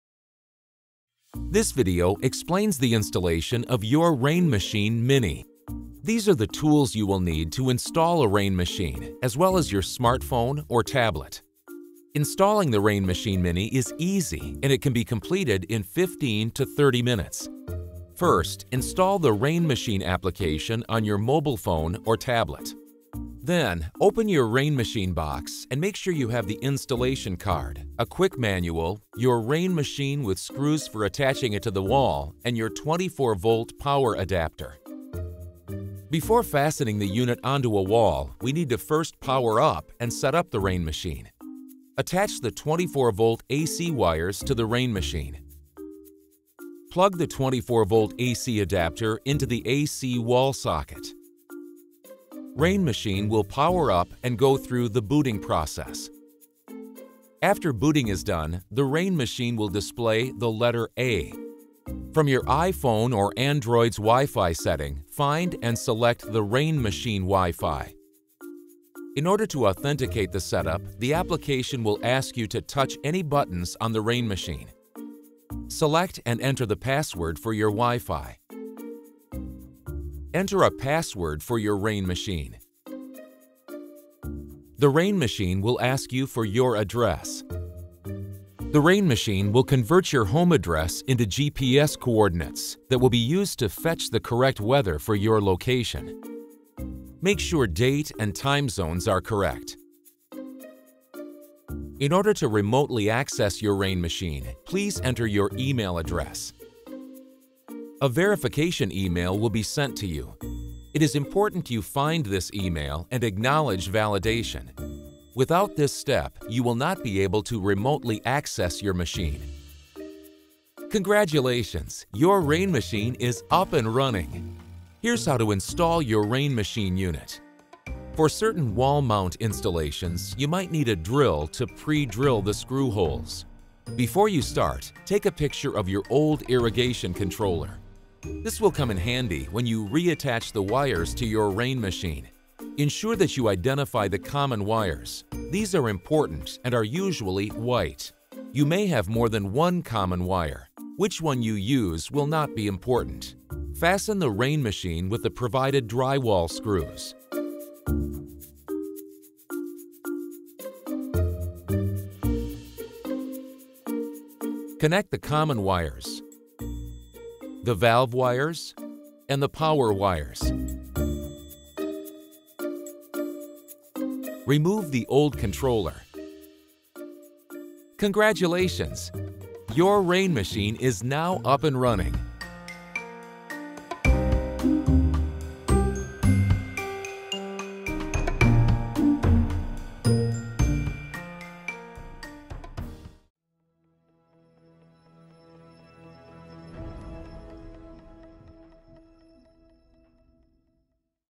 RainMachine Mini-8 Setup
This video explain step by step how to setup the RainMachine Mini-8 Smart WiFi Irrigation Controller from unboxing to up & running.